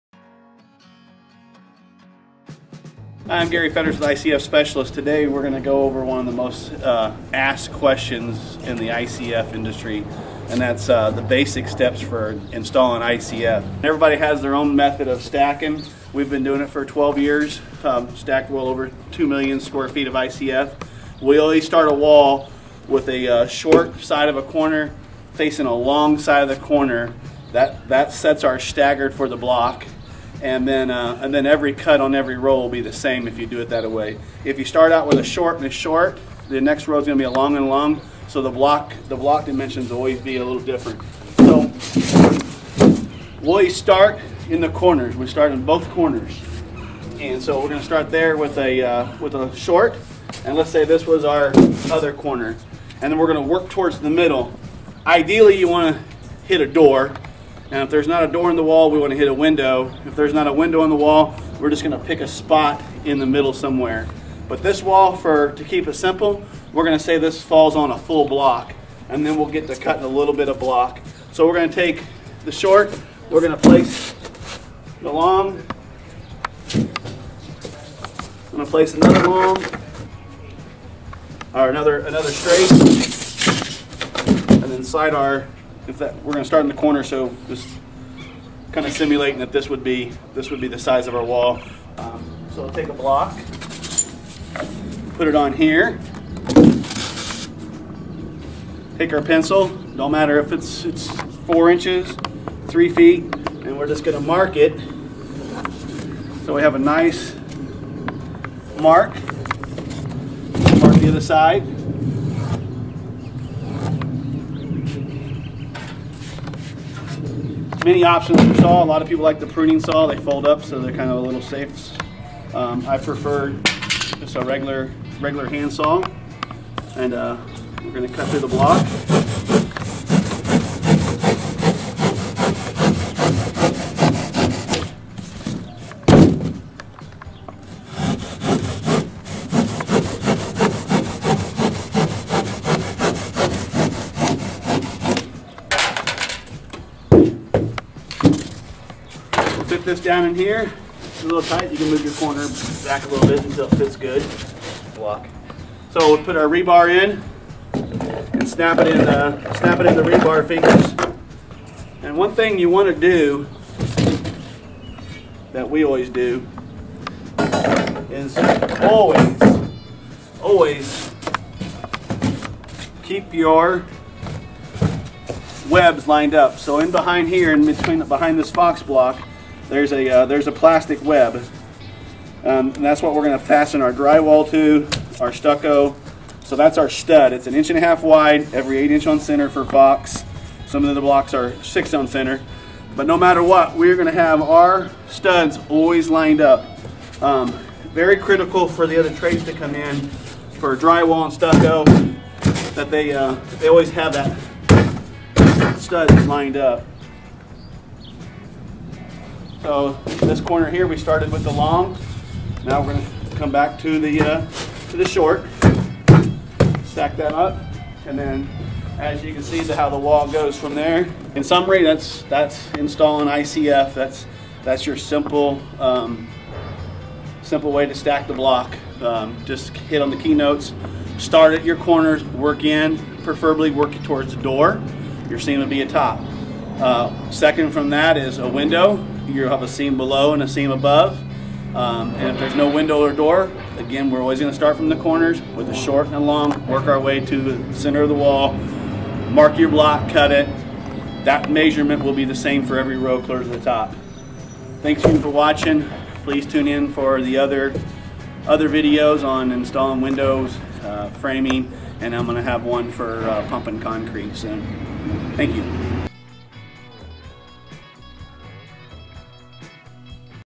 ICF Specialist Step 1 How to Stack ICF
ICF Specialist have been stacking for 12 years and have done over 2 million square feet of ICF

Everyone has their own method for stacking and we are sharing our secret sauce for a successful build

This video covers JUST the stacking of the Insulated Concrete Forms with focus on these steps

Start from the corners
Have short side face a long side
Work your way inward to the center
Mark your block and cut if needed
Stagger your block on each row when stacking
When stacking plan for your windows and doors

We will be covering the other key components in building with ICF in our upcoming videos.

We value your questions about our services and ICF Construction. Thank you!